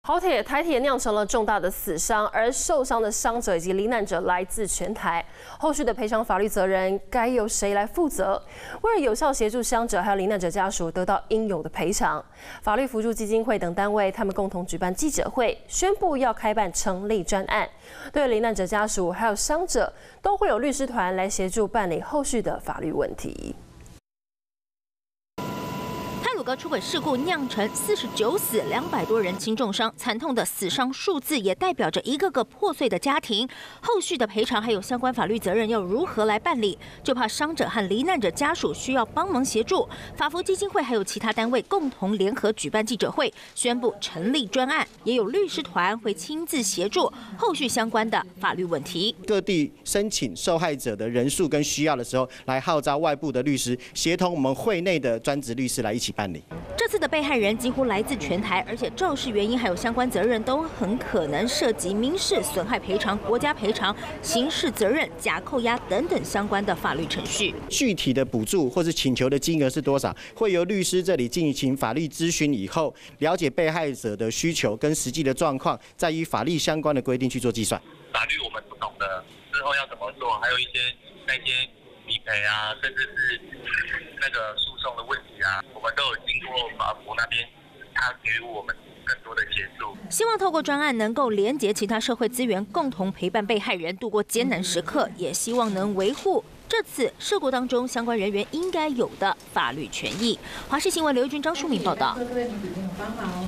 太魯閣號事故 法扶會專案提供法律協助｜華視新聞 20210415
這次台鐵太魯閣出軌事故釀成200多人傷，49人罹難的重大死傷，而受傷罹難者來自全台，後續的賠償法律責任該找誰負責，為了有效協助傷者和罹難者家屬得到應有賠償，法律扶助基金會等單位共同舉辦記者會，宣佈開辦成立專案，對罹難者家屬和傷者不論資力狀況，都會有律師團協助辦理後續法律問題。